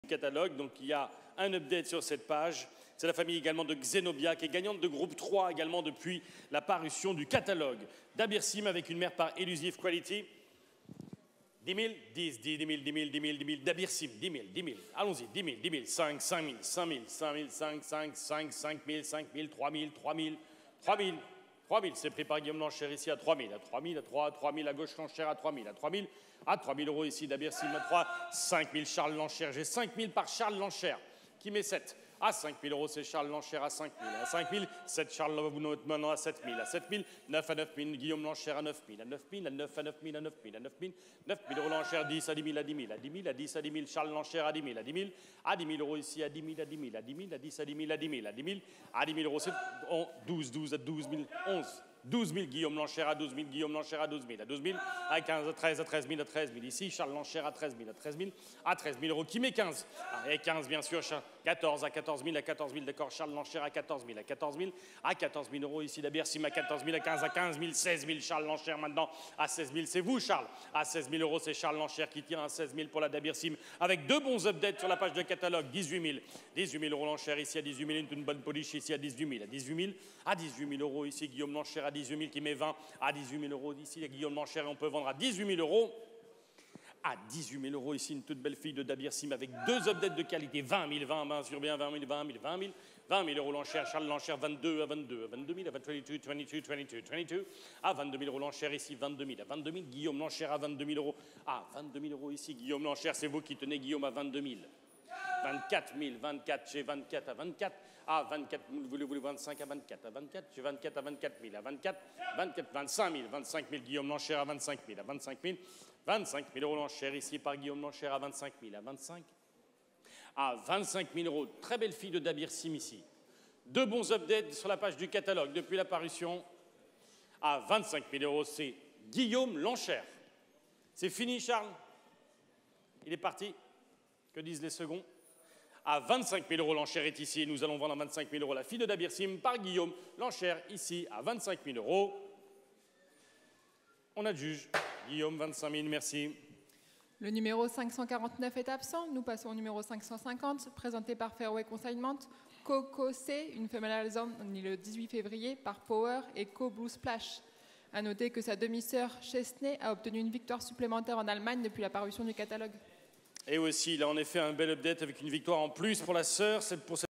Top Price N°2 J4 - 25 000E - N(CHARMING TOUCH 2018) - Vente Arqana de Yearlings d'Octobre 2019
Vente Arqana de Yearlings d'Octobre 2019 - J4
Top Price N°2 - Lot 548
N(CHARMING TOUCH 2018)
25 000E - BOISNARD Joel